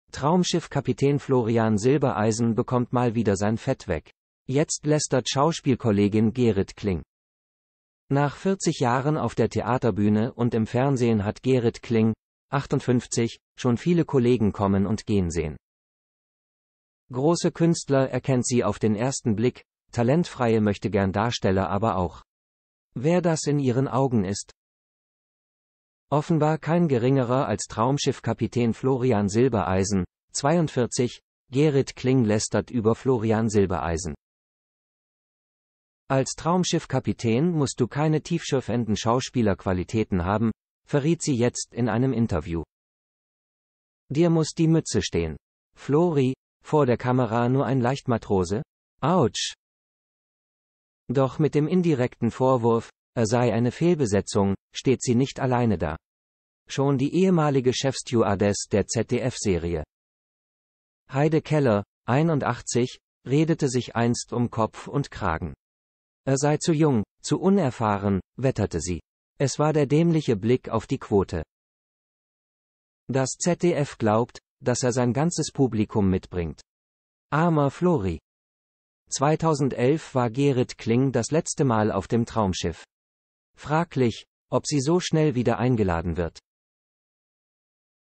Gerit Kling Knallharte Abrechnung mit Florian Silbereisen!
"Traumschiff"-Kapitän Florian Silbereisen bekommt mal wieder sein Fett weg. Jetzt lästert Schauspielkollegin Gerit Kling.